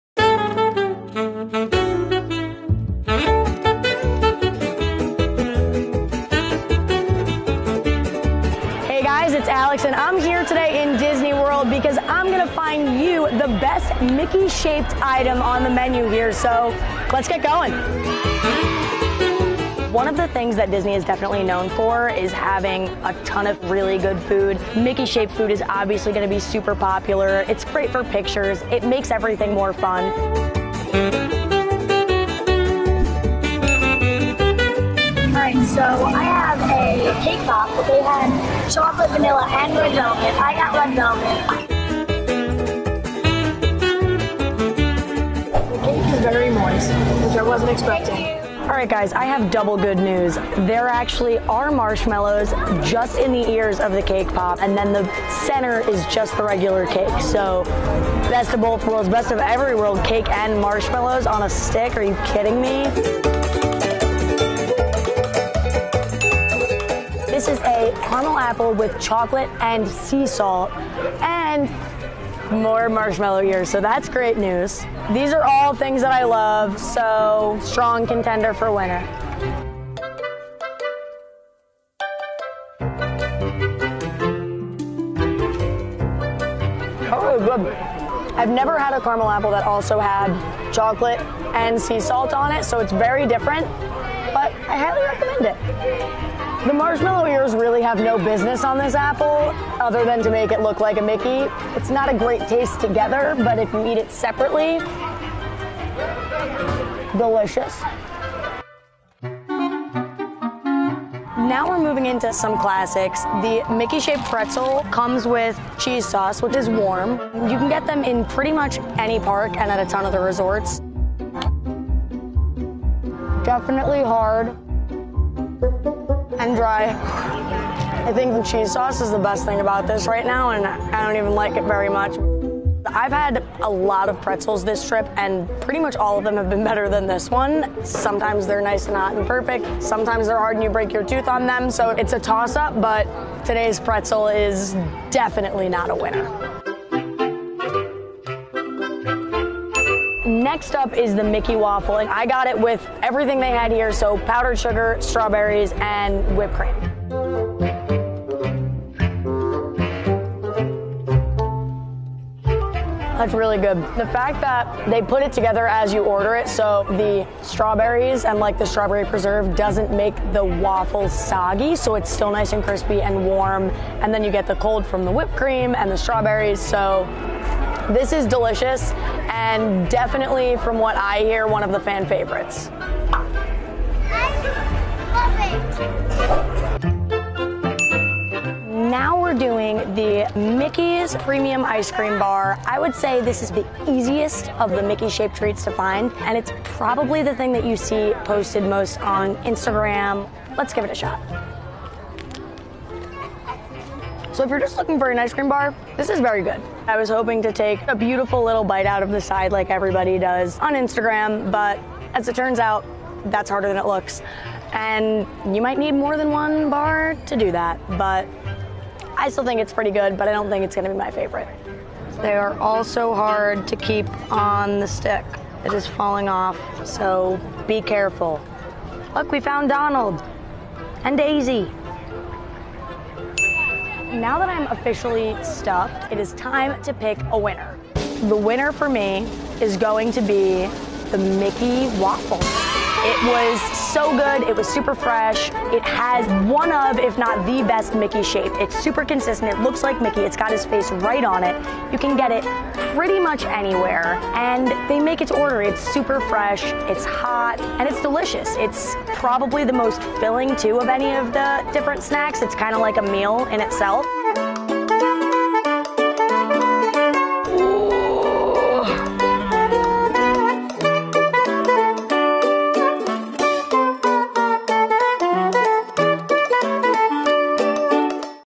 The Best Mickey on the Menu at Disney World
We went to Walt Disney World in Orlando, Florida where we ate our way around the parks — Magic Kingdom, Epcot, Hollywood Studios, and Animal Kingdom — to find the best Mickey-shaped food item to eat.